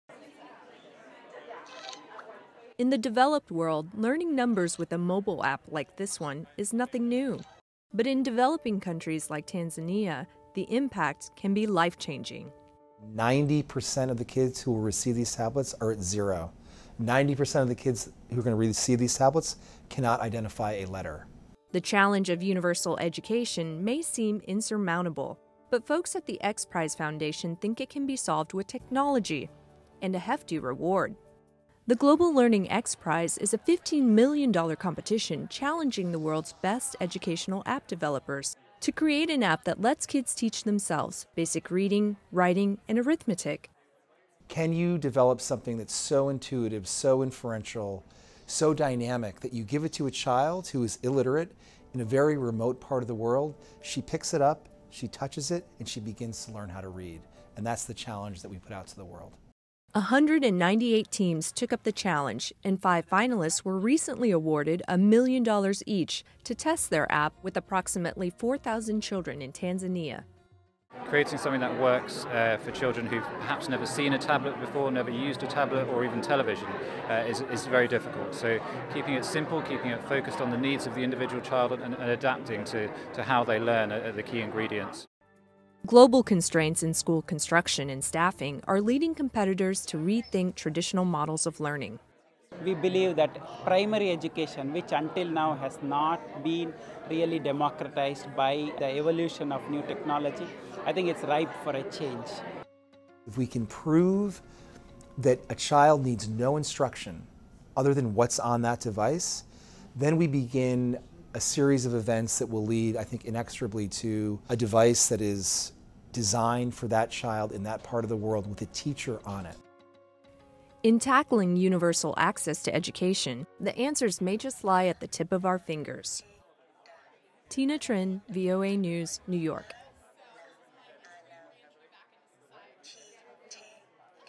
Global Learning XPRIZE Aims to Prove World’s Poorest Children Can Educate Themselves
Can children who have never been to school teach themselves basic reading, writing and math skills using only a tablet computer? Elon Musk and the XPrize Foundation are betting $15 million on the idea that they can. VOA’s Tina Trinh has more.